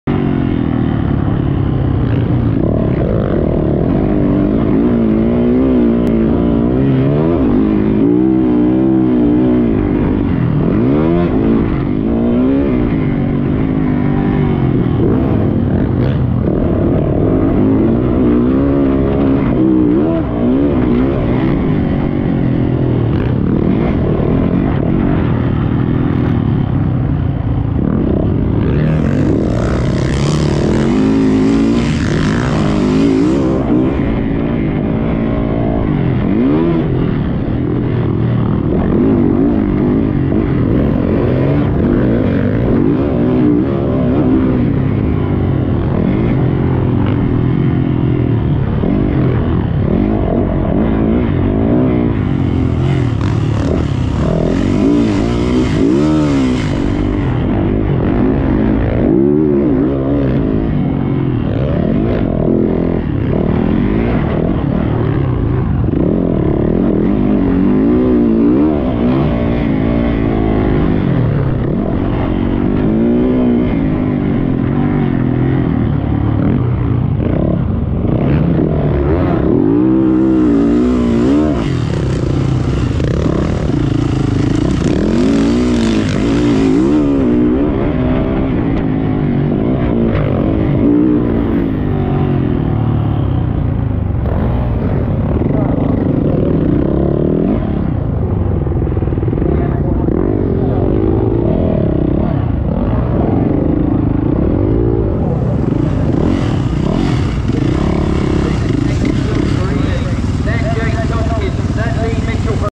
AMCA NATIONAL CHAMPIONSHIP R3 HAWKSTONE PARK mx1 juniors race 1 part 2
Finished 5th after a few tumbles